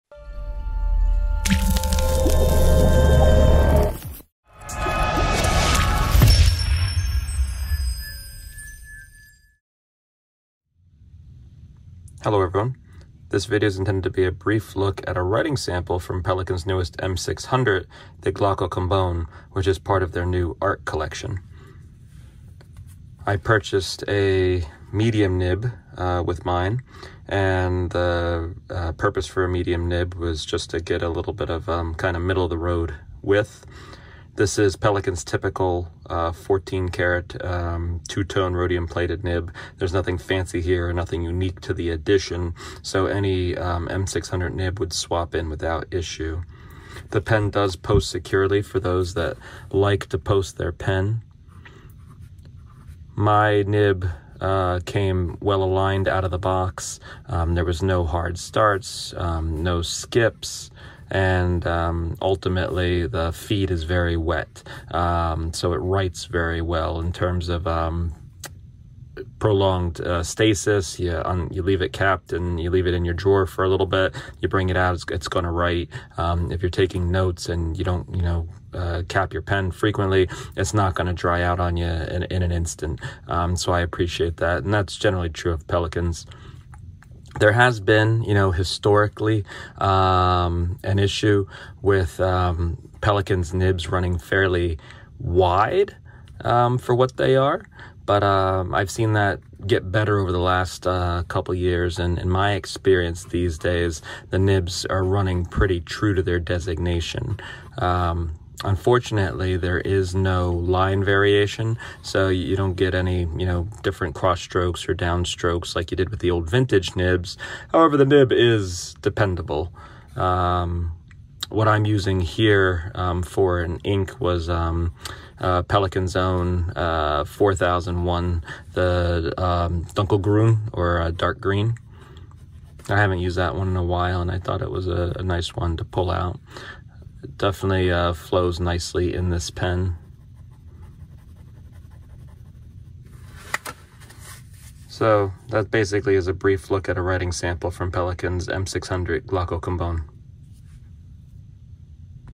Pelikan M600 Art Collection - Glauco Cambon (2023) Writing Sample
A brief look at a writing sample from the Pelikan M600 Art Collection - Glauco Cambon fountain pen (2023) with a 14C-585 gold medium nib.